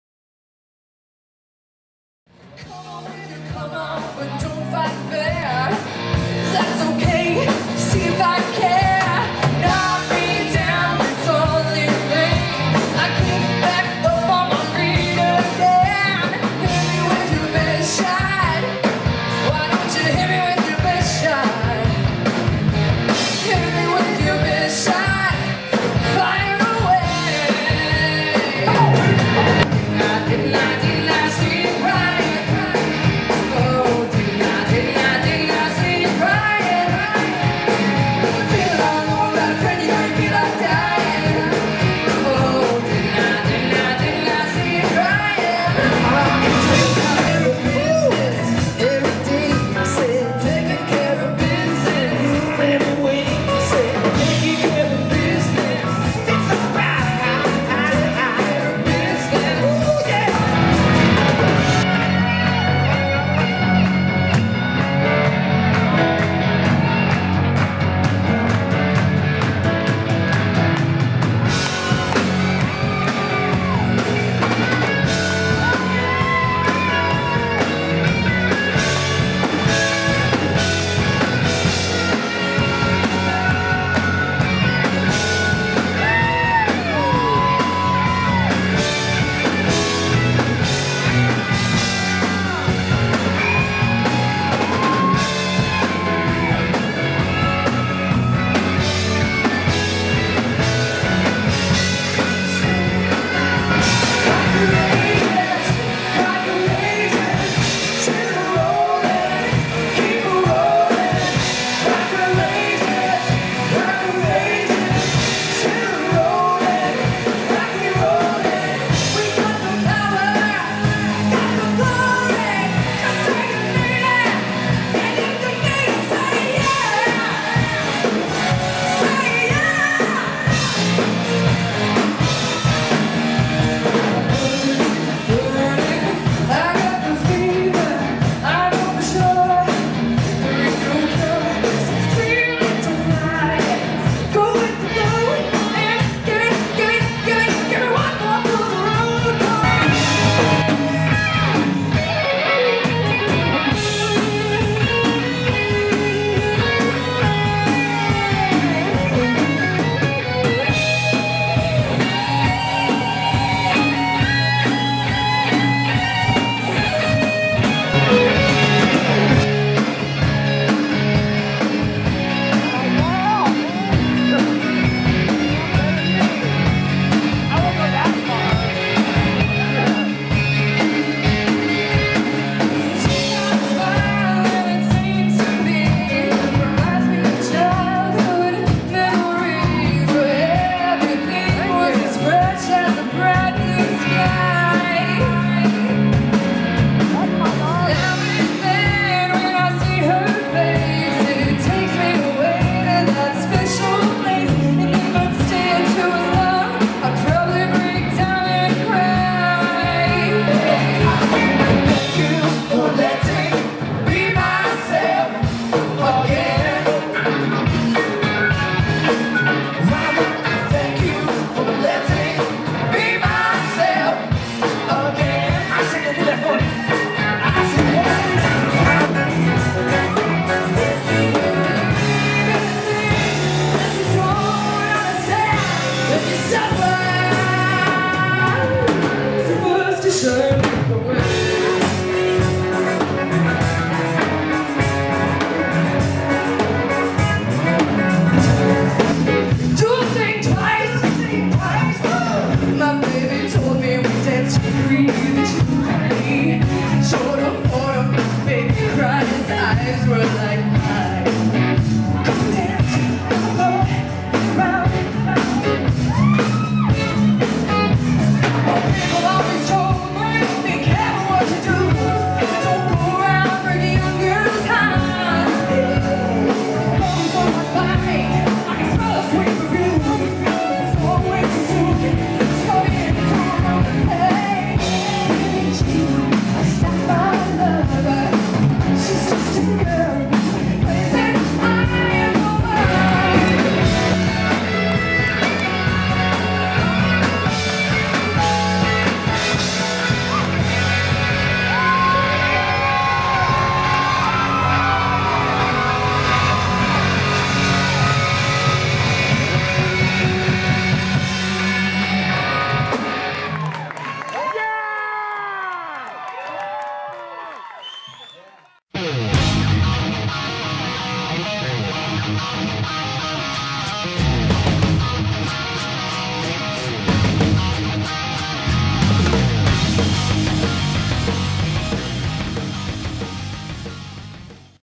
IROC Band - Springfield, Missouri 2012
Compilation video of IROC at Three 20's, November 30th. Most is of my daughter, Casandra... I guess I was a little biased with the camcorder. This was their 1st full show together, and it was absolutely fantastic! They specialize in 80's rock, but throw in some surprises, as well. Weird being on the watching end and her on the performing end- that's all I gotta say!